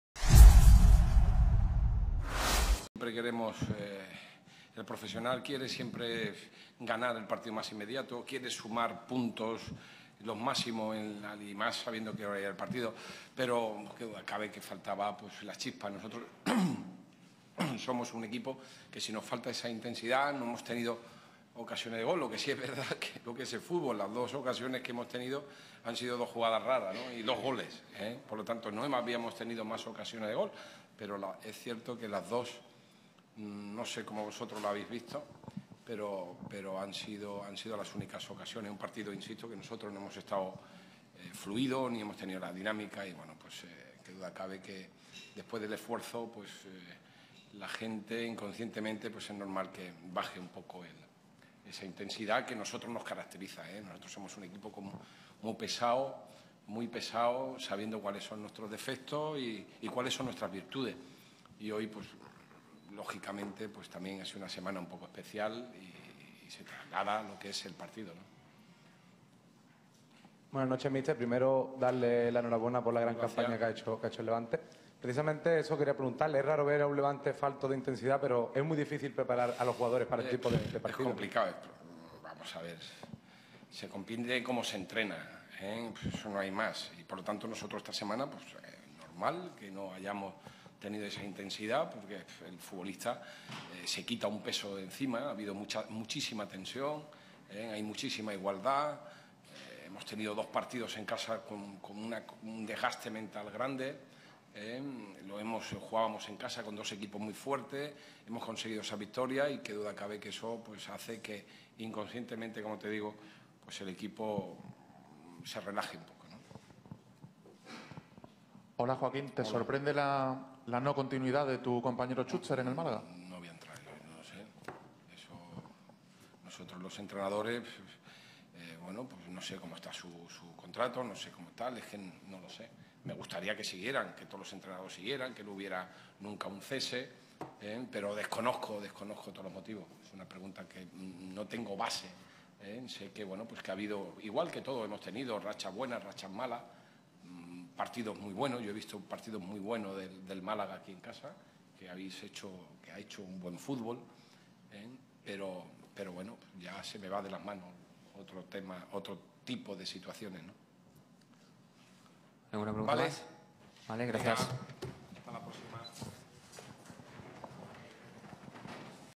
Press Conference Caparrós after Málaga CF (1-0) Levante UD - HD
Press Conference Caparrós after Málaga CF (1-0) Levante UD - HD SUSCRIBETE AL MEJOR CANAL HD La Liga | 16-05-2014 | J38 | Liga BBVA 2013/2014 Press conference Caparrós after Málaga CF (1-0) Levante UD 38th Round La Liga 2013/2014 Youtube